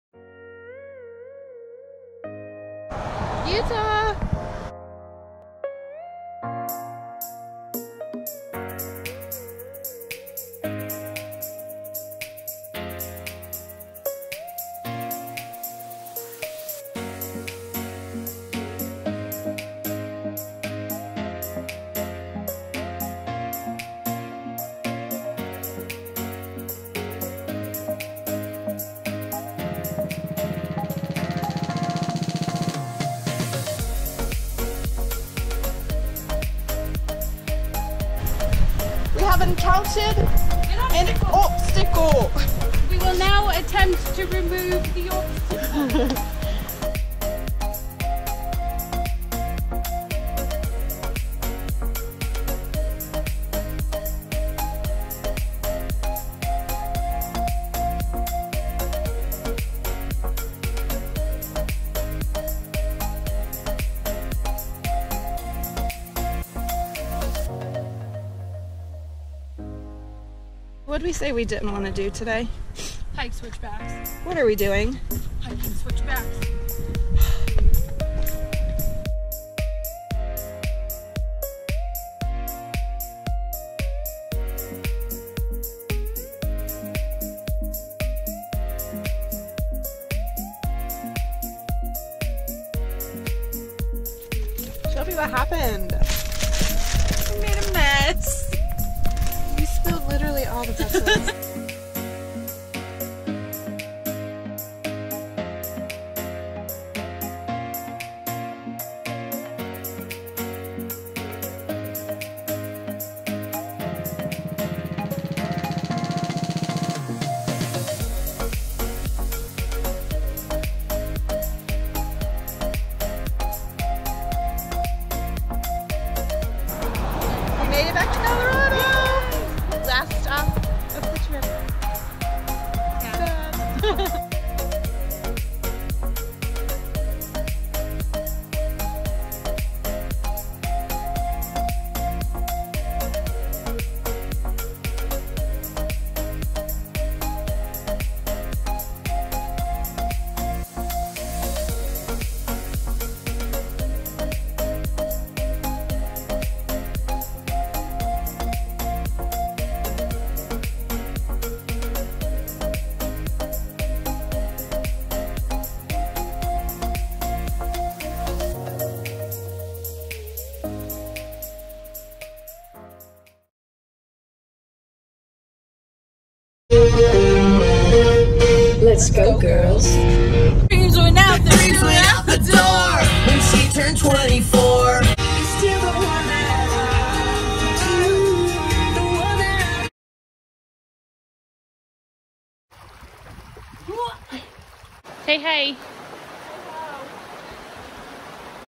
National Park Roadtrip 2020 Travel Video Part 3
This video is shot in Utah and Colorado! My best friend and I took a 10 day trip out West to visit some of the most beautiful National Parks and National Recreation Areas!

Our adventures consisted of exploring Salt Lake City, refreshing hikes along a stream in Capitol Reef, hikes to Arches (not at Arches NP,) switchbacks we didn't want to hike, hikes to Arches (in Arches NP,) windy Canyonlands Hikes, singing in the car, hitting a larges questionable thing on the highway ... and a beautiful black canyon.